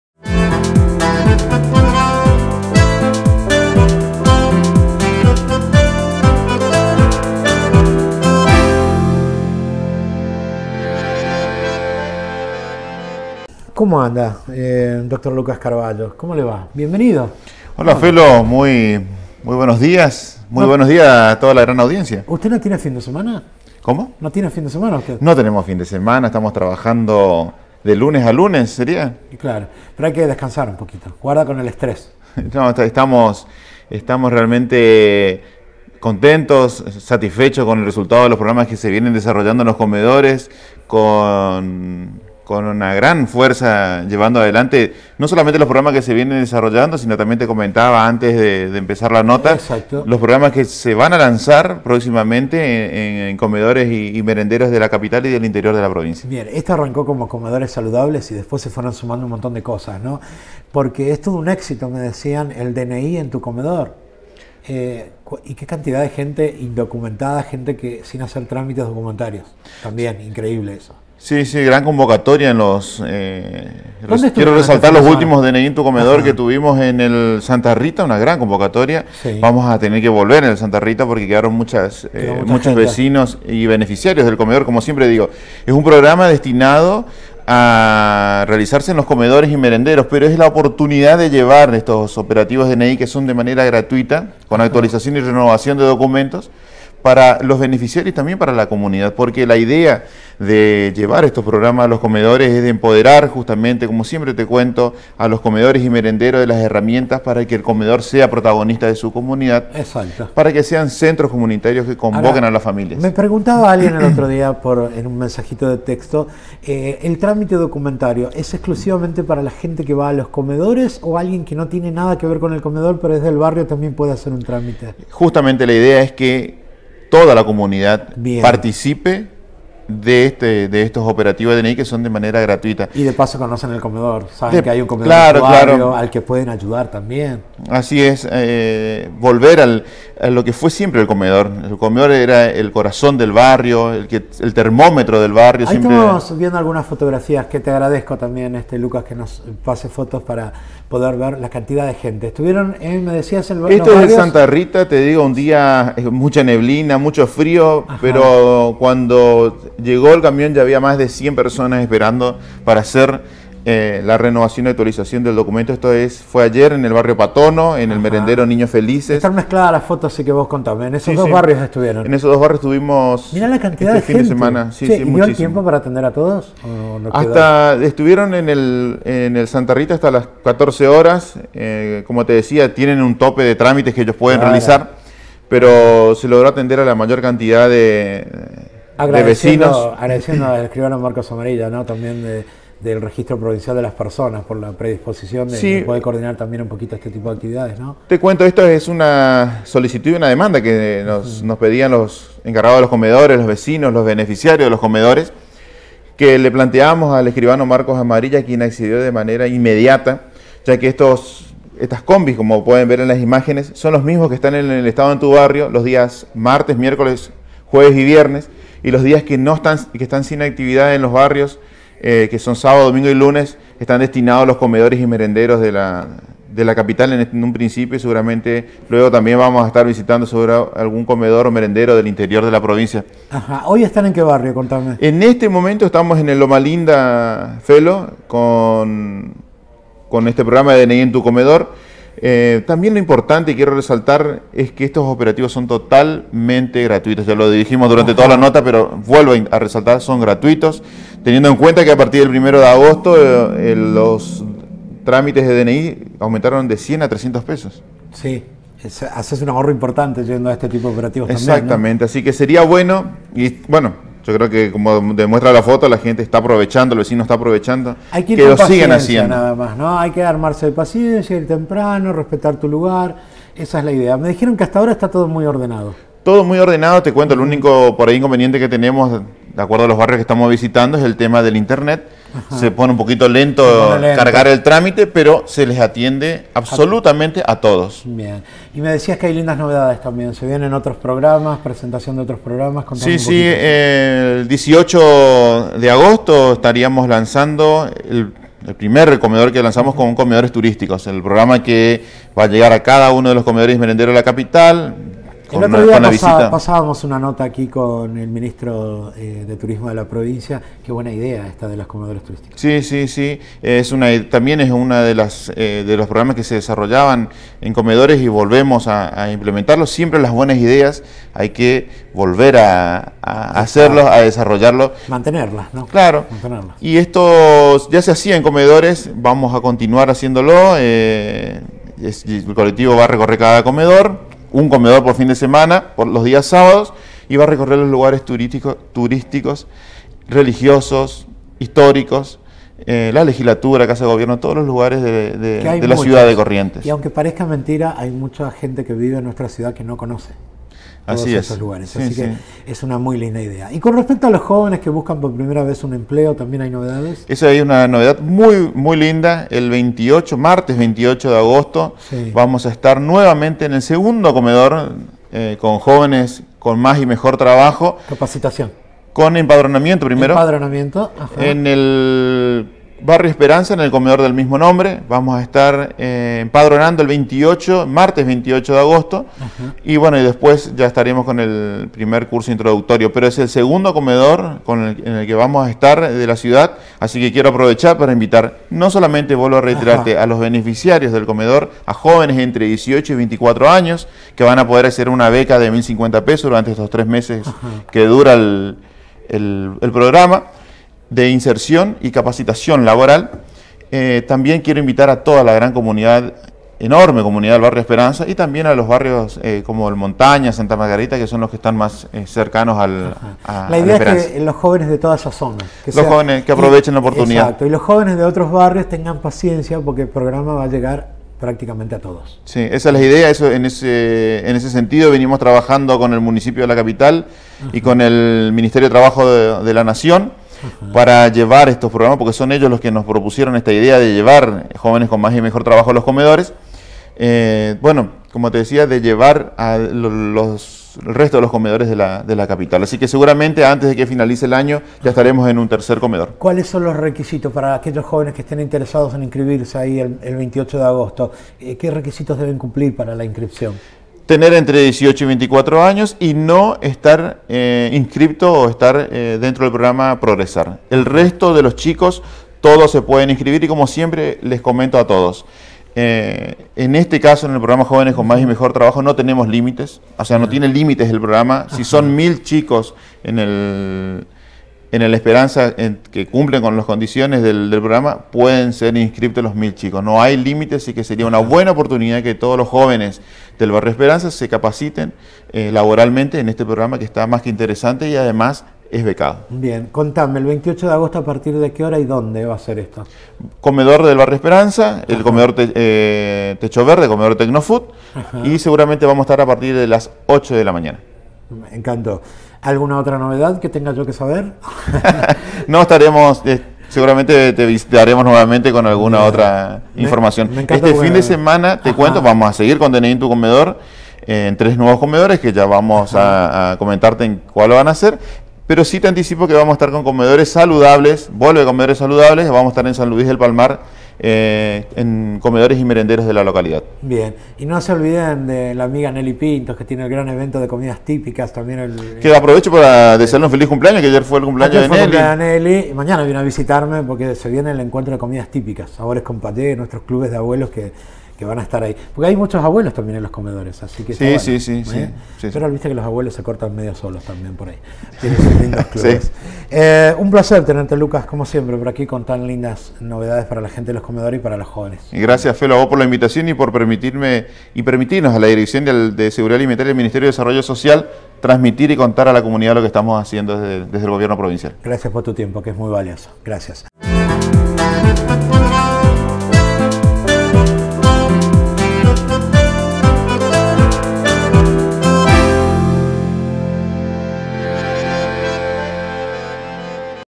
Dr LUCAS CARBALLO
Hoy nos visita El Doctor Lucas Carballo (Director de Seguridad Alimentaria del MDS) para comentar un poco a la comunidad sobre el DNI en tu comedor.